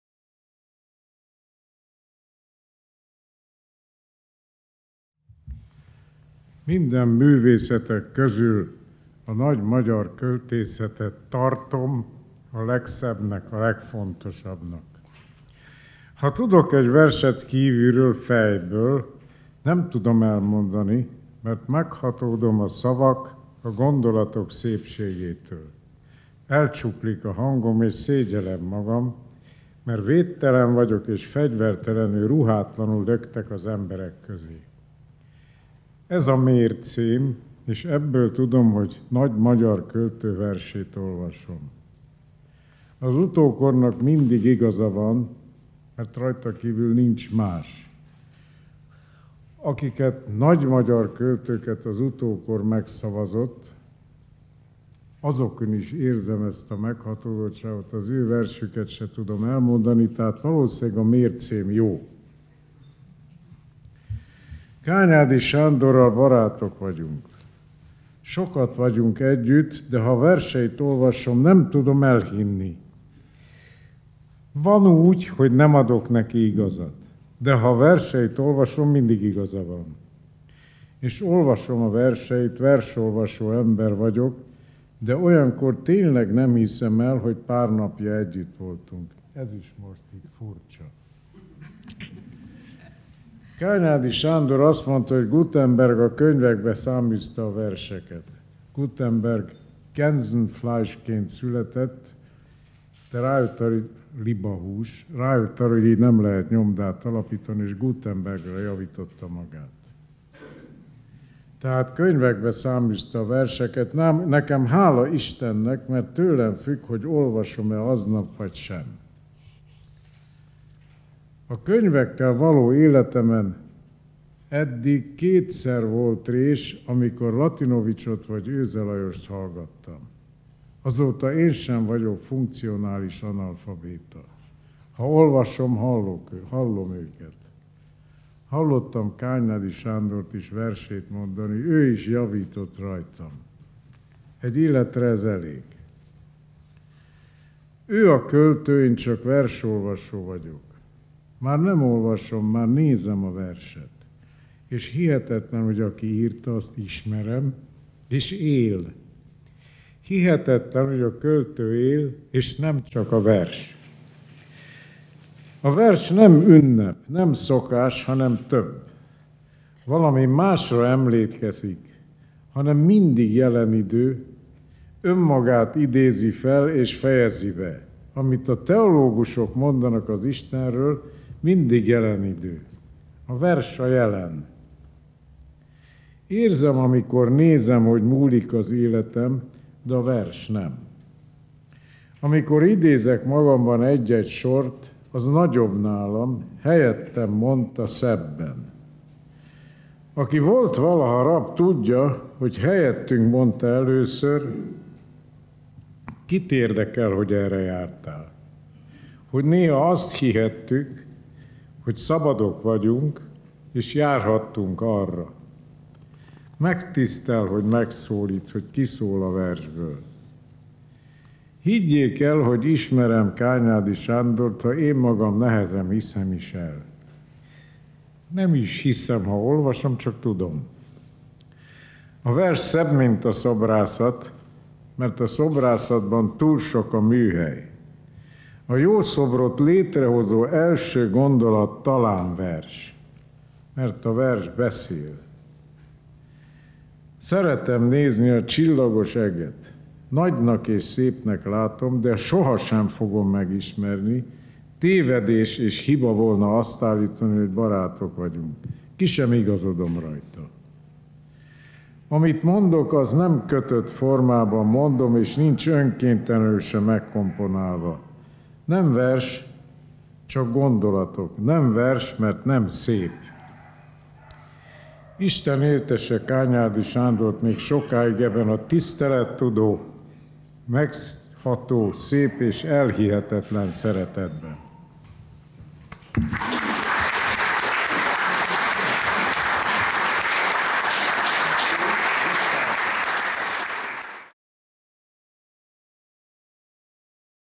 Kányádi Sándort köszönti 85. születésnapja alkalmából Melocco Miklós szobrászművész
Melocco Miklós szobrászművész, régi barátként köszöntötte akadémikus társát, Kányádi Sándor költőt, akinek 85. születésnapja alkalmából zenés irodalmi estet rendeztek 2014. május 13-án a Petőfi Irodalmi Múzeumban. A szobrászművész kiemelte: „Minden művészetek közül a magyar költészetet tartom a legszebbnek, a legfontosabbnak". Melocco Miklós elmondta, hogy hiába tud fejből egy verset, elmondani képtelen, mert meghatódik a szavak, a gondolatok szépségétől. Úgy folytatta, hogy ők Kányádi Sándorral barátok és sok időt töltenek együtt, verseit olvasva mégis gyakran megdöbben, hogy ismeri annak ma élő szerzőjét.